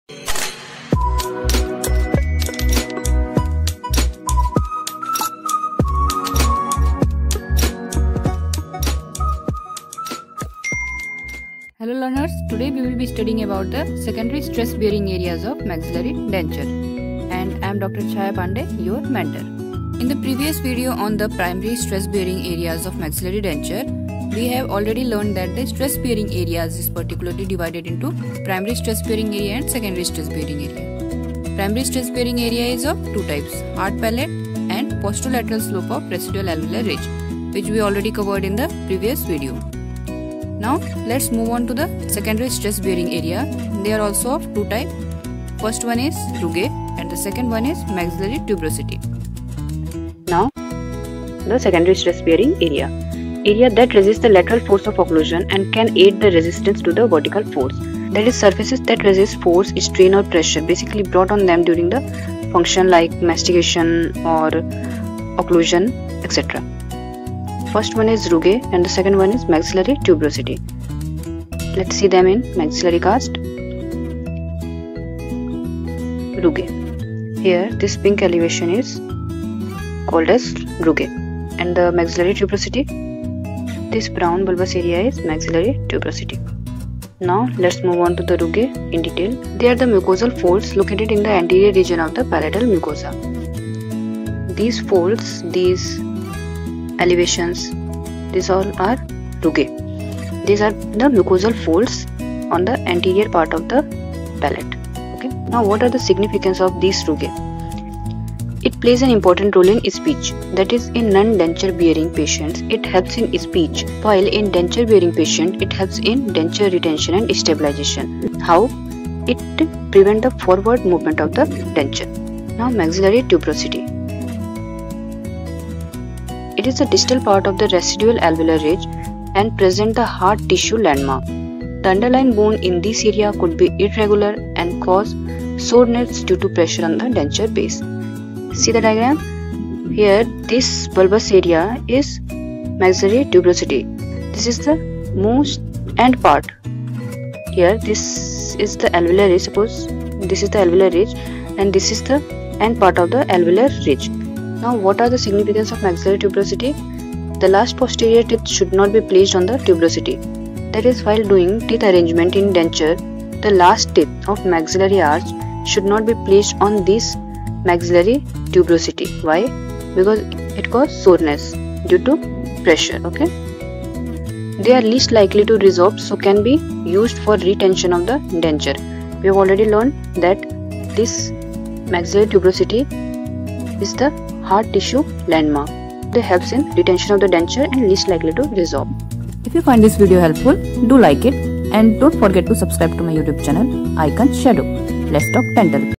drchhaya Secondary stress bearing area of maxilla|anatomical landmarks|rugae|maxillary tuberosity
IN THIS VIDEO WE WILL LEARN ABOUT THE SECONDARY STRESS BEARING AREAS OF MAXILLARY DENTURE.

STRESS BEARING AREAS ARE BASICALLY OF TWO TYPES
1) PRIMARY STRESS BEARING AREAS
2) SECONDS STRESS BEARING AREAS

PRIMARY STRESS BEARING AREAS ARE:-
A) HARD PALATE
B) POSTERO-LATERAL SLOPE OF RESIDUAL ALVEOLAR RIDGE.

SECONDARY STRESS BEARING AREAS ARE:-
A) RUGAE
B) MAXILLARY TUBEROSITY

STAY SAFE...😷

music credit-